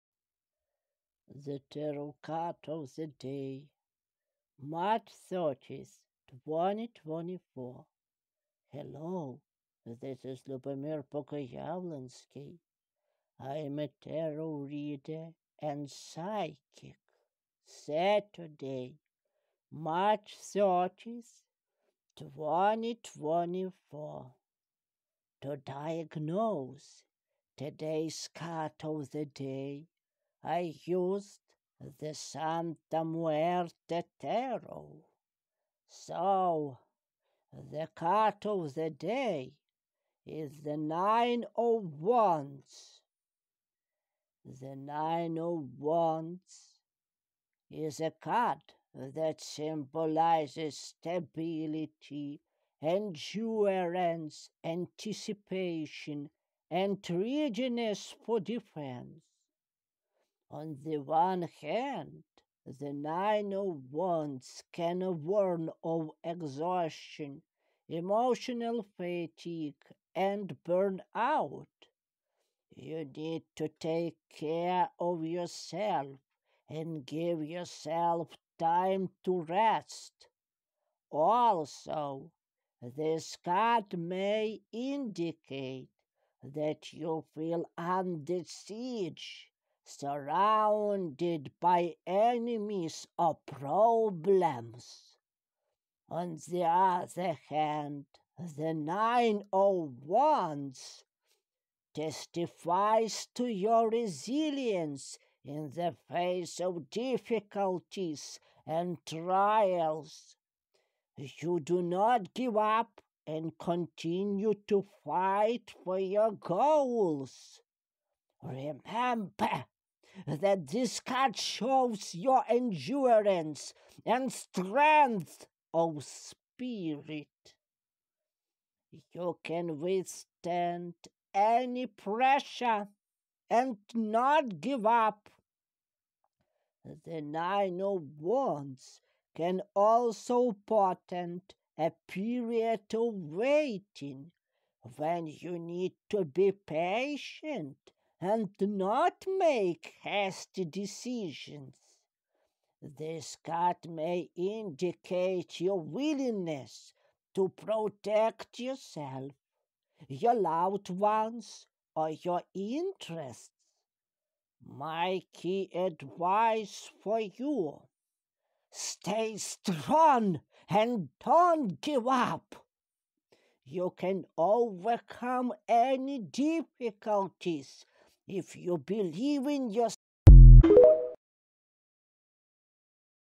The Tarot Card of the Day. March 30, 2024
Hello!
This is Lubomyr Bogoyavlensky. I’m a Tarot reader and psychic.
Saturday, March 30, 2024. To diagnose today's card of the day, I used the Santa Muerte Tarot. So, the card of the day is the Nine of Wands.
The Nine of Wands is a card that symbolizes stability, endurance, anticipation and readiness for defense.
On the one hand, the Nine of Wands can warn of exhaustion, emotional fatigue and burnout. You need to take care of yourself and give yourself time to rest.
Also, this card may indicate that you feel under siege, surrounded by enemies or problems.
On the other hand, the Nine of Wands testifies to your resilience in the face of difficulties and trials. You do not give up and continue to fight for your goals.
Remember that this card shows your endurance and strength of spirit. You can withstand any pressure and not give up.
The Nine of Wands can also portend a period of waiting, when you need to be patient and not make hasty decisions.
This card may indicate your willingness to protect yourself, your loved ones, or your interests.
My key advice for you: Stay strong and don't give up. You can overcome any difficulties if you believe in yourself and don't give up.
Sincerely, your Tarot reader Lubomyr Bogoyavlensky